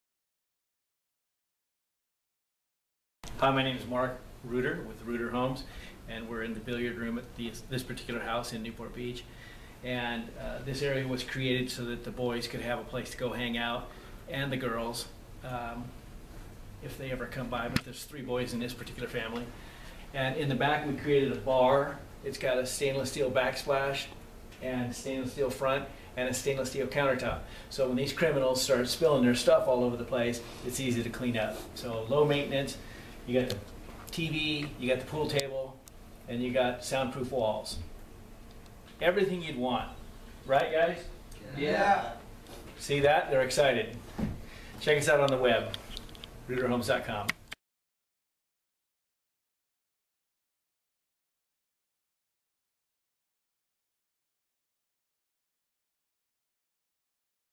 Custom TV rooms and entertainment rooms in your home by Reuter Homes
Orange County custom built TV rooms, entertainment rooms and billiard rooms by Reuter Homes.